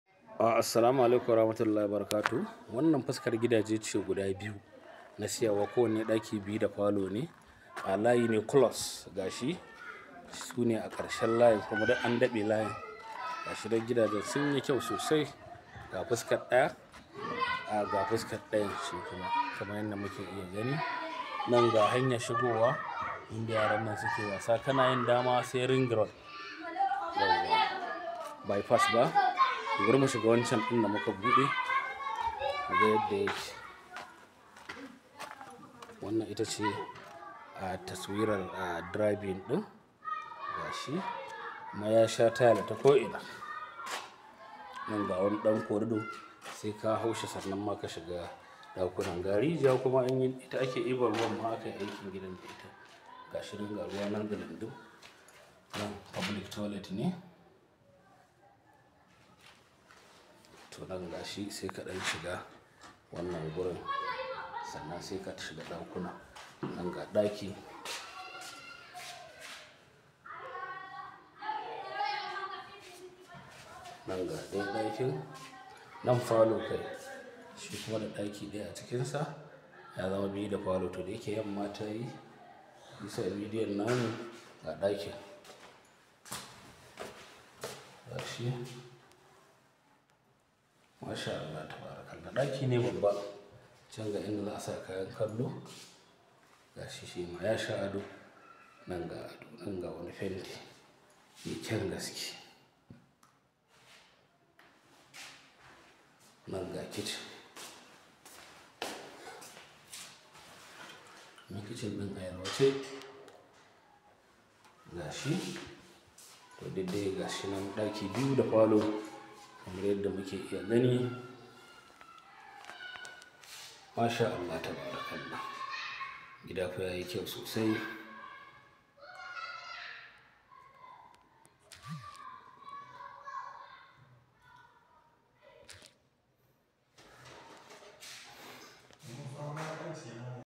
₦5.5 million Unguwar Jakada Ring Road, Kano State Nigeria. 08093124083 /09066073407
₦5.5 million Unguwar Jakada Ring Road, Kano State Nigeria. 2 bedrooms and living room.

Domin neman ƙarin bayani a tuntuɓi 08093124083 /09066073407